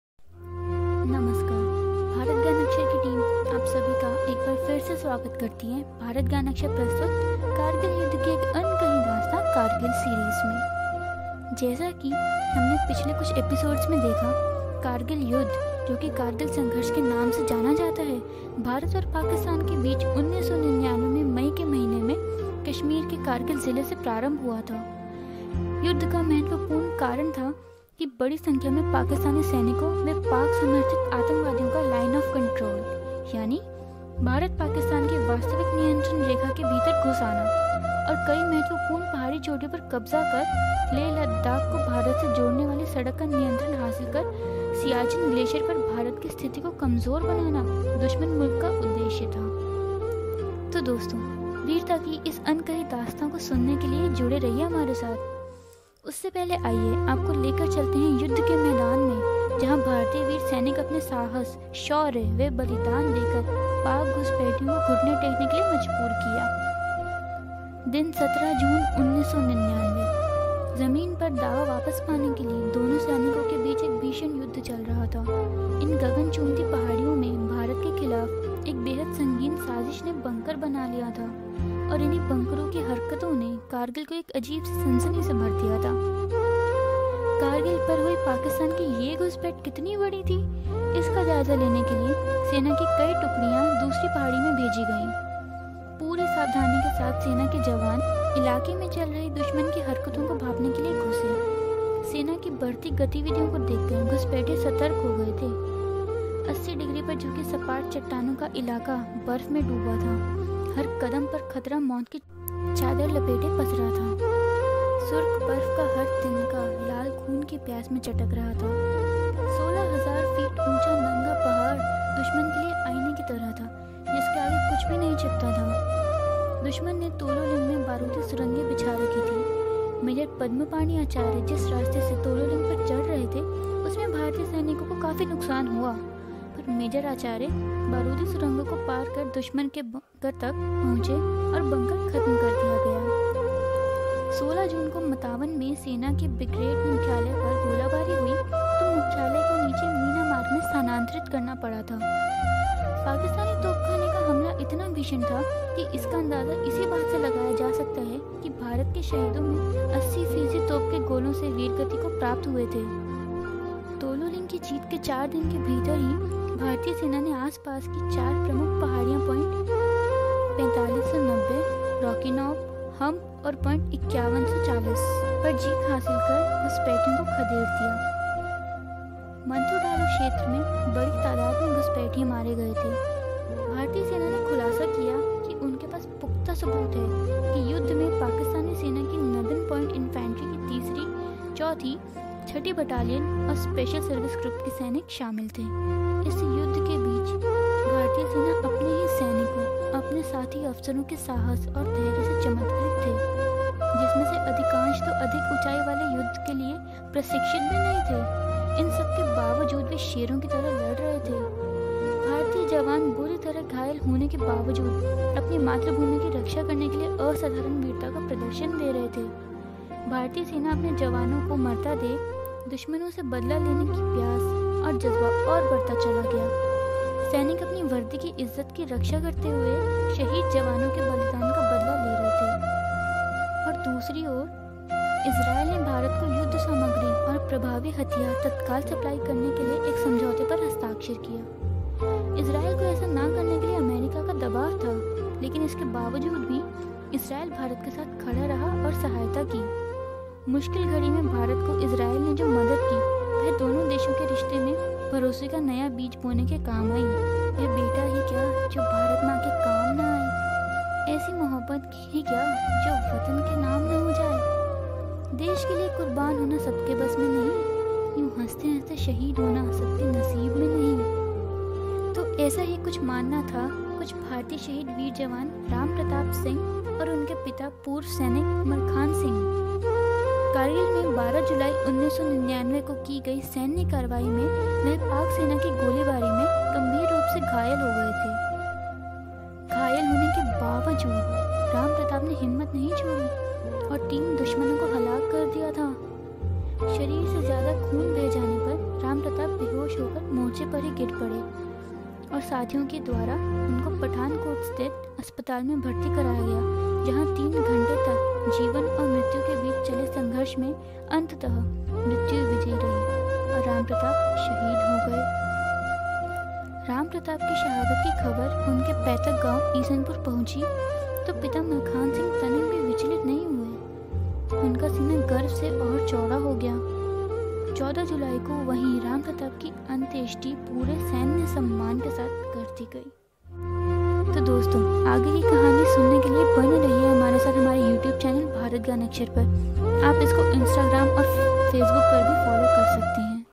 Kargil War - जीत का ज़ज्बा | Kargil Martyr RamPratap Singh | Kargil Series
Kargil War - जीत का ज़ज्बा | Kargil Shahid Ram Pratap Singh (शहीद राम प्रताप सिंह) || KargilSeries || Bharat Gyanakshar|| Bg Info Media presents

this series is an initiative of Bharat Gyanakshar titled "एक अनकही कहानी: कारगिल युद्ध सीरीज'' or "An Untold Story: Kargil War Series''. You will also learn about at least one of the martyred soldiers that gave up their life in the war.

Presents: Bharat Gyanakshar
Founder & Director: Sonu Aryan Choubey
Content, Voice and Video Credit: Team Bharat Gyanakshar
SEO Analyst: Suraj Raj Eqbal